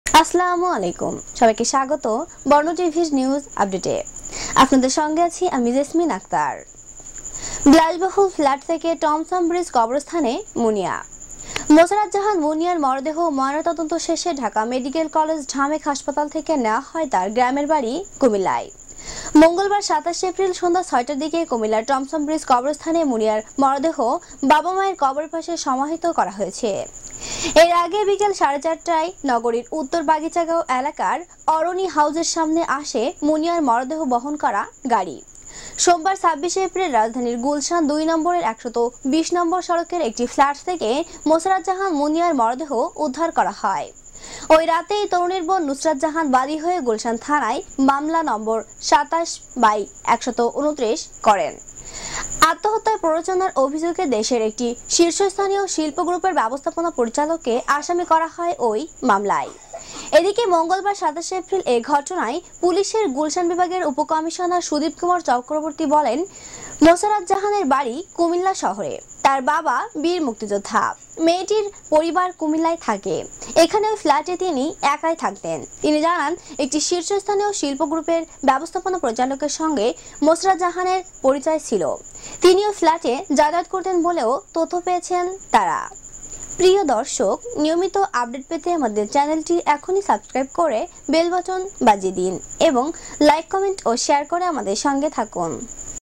বিলাসবহুল ফ্ল্যাট থেকে কবরস্থানে শুয়ে আছে মুনিয়া | আপডেট খবর | প্রতিদিনের সংবাদ | মৃত্যুর বাংলা নিউজ
বিলাসবহুল ফ্ল্যাট থেকে টমছমব্রিজ কবরস্থানে মুনিয়া
  মোসারাত জাহান মুনিয়ার মরদেহ ময়নাতদন্ত শেষে ঢাকা মেডিকেল কলেজ (ঢামেক) হাসপাতাল থেকে নেয়া হয় তার গ্রামের বাড়ি কুমিল্লায়। মঙ্গলবার (২৭ এপ্রিল) সন্ধ্যা ৬টার দিকে কুমিল্লার টমছম ব্রিজ কবরস্থানে মুনিয়ার মরদেহ বাবা-মায়ের কবরের পাশে সমাহিত করা হয়েছে।এর আগে বিকেল সাড়ে ৪ টায় নগরীর উত্তর বাগিচাগাঁও এলাকার অরণি হাউজের সামনে আসে মুনিয়ার মরদেহ বহন করা গাড়ি। সোমবার (২৬ এপ্রিল) রাজধানীর গুলশান ২ নম্বরের ১২০ নম্বর সড়কের একটি ফ্ল্যাট থেকে মোসারাত জাহান মুনিয়ার মরদেহ উদ্ধার করা হয়। ওই রাতেই তরুণীর বোন নুসরাত জাহান বাদী হয়ে গুলশান থানায় মামলা (নম্বর ২৭/১২৯) করেন। আত্মহত্যায় প্ররোচনার অভিযোগে দেশের একটি শীর্ষস্থানীয় শিল্প গ্রুপের ব্যবস্থাপনা পরিচালককে আসামি করা হয় ওই মামলায়।এদিকে মঙ্গলবার (২৭ এপ্রিল) এ ঘটনায় পুলিশের গুলশান বিভাগের উপ-কমিশনার সুদীপ কুমার চক্রবর্তী জানান, মোসারাত জাহানের বাড়ি কুমিল্লা শহরে। তার বাবা বীর মুক্তিযোদ্ধা। মেয়েটির পরিবার কুমিল্লায় থাকে। এখানে ওই ফ্ল্যাটে তিনি একাই থাকতেন। তিনি জানান, একটি শীর্ষস্থানীয় শিল্প গ্রুপের ব্যবস্থাপনা পরিচালকের সঙ্গে মোসারাত জাহানের পরিচয় ছিল। তিনি ওই ফ্ল্যাটে যাতায়াত করতেন বলেও তথ্য পেয়েছেন তারা।